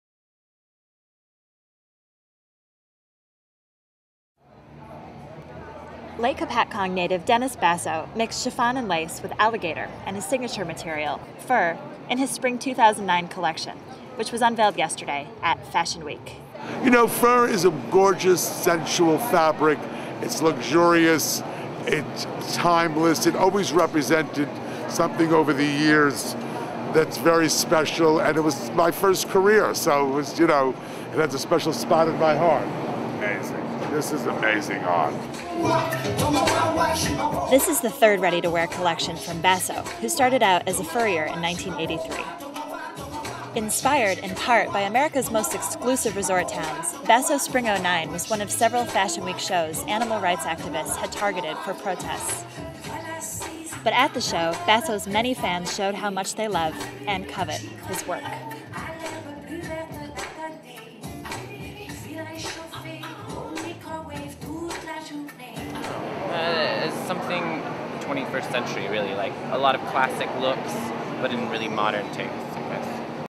Dennis Basso's Spring 2009 collection
Inspired by sun, sea, exclusive resorts and boho looks of the early-1970s, Dennis Basso's Spring 2009 collection hit the runway yesterday at Fashion Week. Basso, who has worked with fur for 25 years, included broadtail in the collection. Broadtail is the fur of stillborn lambs, according to the Fur Information Council of America; the Humane Society of the United States says it's the fur of fetal lambs. Animal-rights activists had planned to protest his show, but inside the tents, the mood was largely adoring.